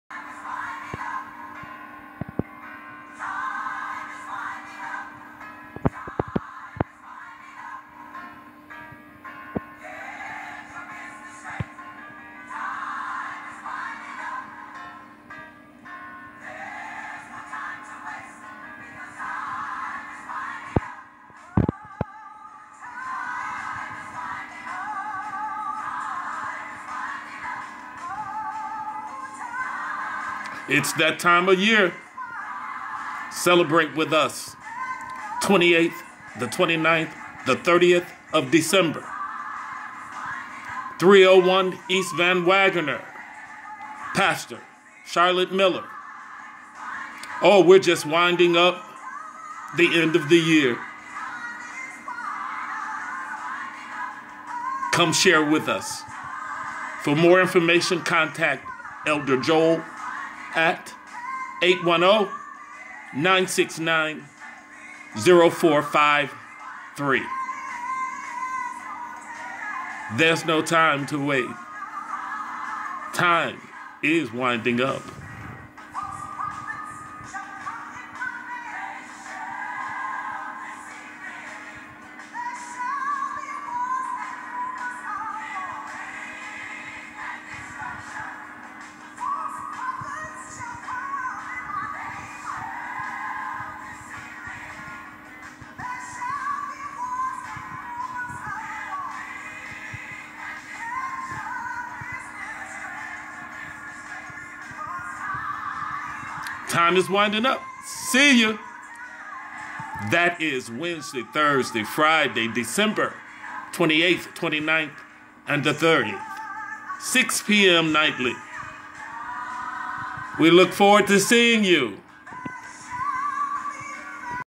Time is winding up. Get your business straight no time to hesitate.
The time for gathering with family, friends, co laborers as we celebrate the new born King Jesus.

You are invited to share in our Holiday Prayer Praise and Worship  Services.

December 28th 29th and 30th 2017